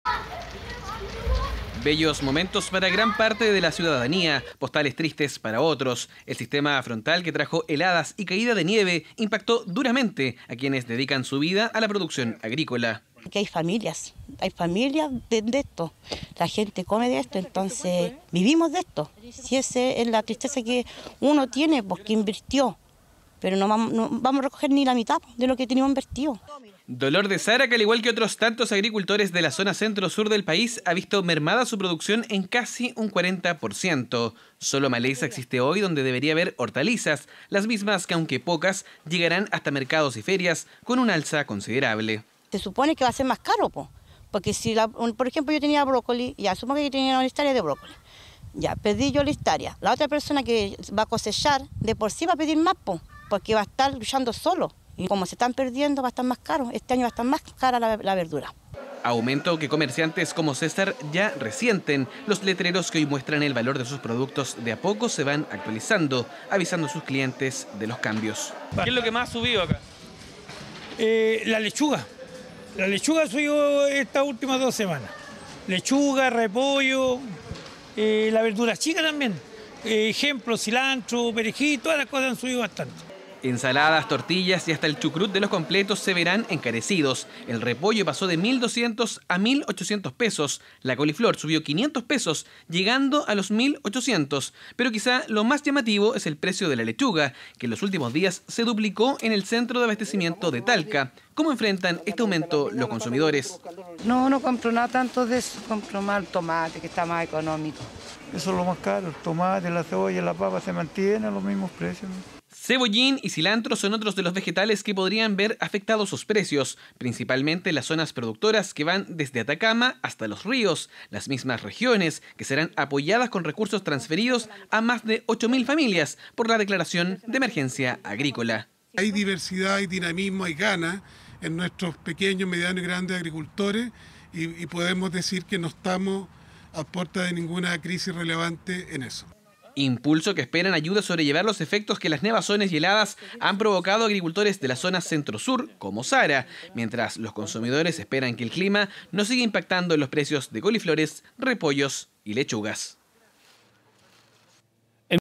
Precio de lechugas se duplicó en el Maule tras nieve y heladas
Las recientes lluvias y caída de nieve en distintas zonas del país trajo alegría, pero también consecuencias. En la región del Maule las heladas afectaron algunos cultivos. Las hortalizas aumentaron su precio, el valor de la lechuga incluso se duplicó.